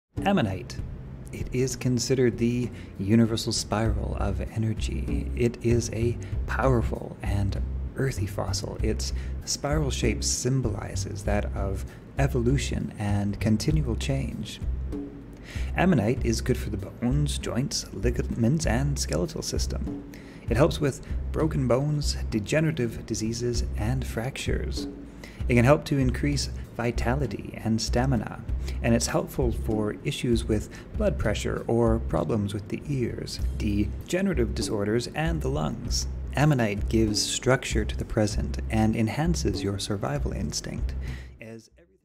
Ammonite Meaning Benefits and Spiritual Properties
Enroll FREE for 14 Days

Chapters:

Ammonite Meaning and Spiritual Properties

Ammonite is considered the universal spiral of energy. It is a powerful and earthy fossil. Its spiral shape symbolizes that of evolution and continual change.

Physical Energy Properties: Ammonite is good for the bones, joints, ligaments and skeletal system. It helps with broken bones, degenerative diseases, fractures and osteoporosis and osteoarthritis. It can help to increase vitality and stamina. Helpful for issues with blood pressure or problems with the ears, degenerative disorders and lungs.

Emotional Energy Properties: Ammonite gives structure to the present, and enhances your survival instinct. As everything goes around in cycles, resolving the past helps in the present, with the past helping the future and solving problems before they arise. It is good for creative careers. It may also help to lift the heaviness that can sometimes be felt during depression.

Spiritual Energy Properties: Ammonite is good for connecting with your ancestors and with past lives. This stone has absorbed cosmic energy over eons of time and helped to stimulate the life force (Chi) within. It is often used for activating Kundalini and life path energies.

Chakras: Base, Heart

Astrological signs: Aquarius

History: Ammonite stone is a fossilized sea animal that lived 65 to 240 million years ago. This sea animal became extinct when the dinosaurs went extinct.

Source: Worldwide.

Variations, Rarity and Value: Ammonite is found in every natural color. Iridescent Ammonites may often be of gem quality when polished. This iridescence would not have been visible during the animal's life as additional shell layers covered it.

How to use it: The Ammonite is also often recommended to put in the home to attract balance, prosperity and success. It is good for creative careers.